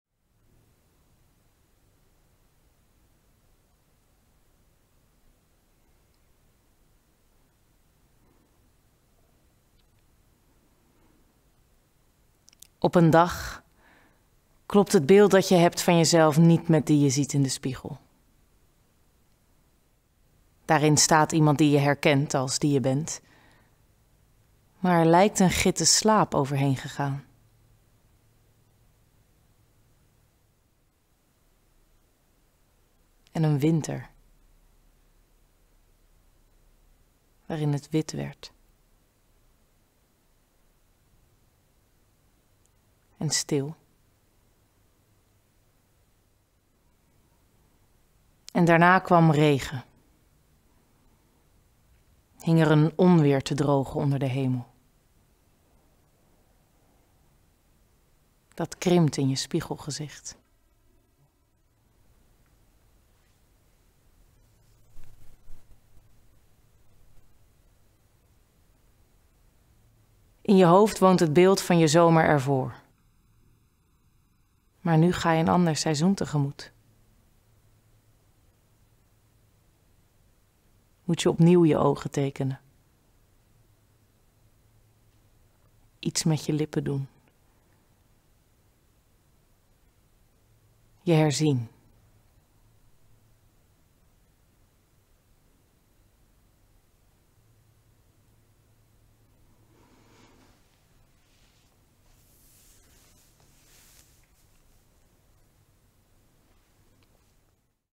Herfstkus 25 september - Toneelgroep Maastricht
Claire Hordijk draagt 'Het hebben van plaats' van Hester Knibbe voor.

Van dinsdag 21 tot en met zondag 26 september 2021 deelt Toneelgroep Maastricht zes ochtenden lang bij zonsopgang gratis herfstkusjes uit. Herfstkus, bedacht en gemaakt door Zorba Huisman, is een serie van zes korte filmpjes. Zes acteurs dragen in een monoloog korte teksten of poëzie voor die in het teken staan van het aanbreken van het nieuwe seizoen. Herfstkus is de opvolger van de succesvolle Lentekus en Zomerkus. De cameravoering is van Coen Leuven.